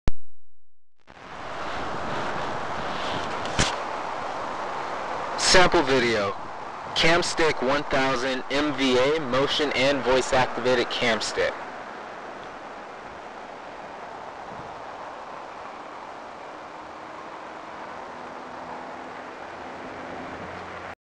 CamStick1000MVA -- Motion and Voice Activated Camstick -- Demo Video
Technical Specs
Video Resolution: 640 x 480
Video Format: AVI
Storage: MicroSD card up to 32GB
Battery Life: Approx 90 minutes continuous recording and 8 days standby
Infrared Body Detector Range: 3 meters
Camera Angle: 68 Degrees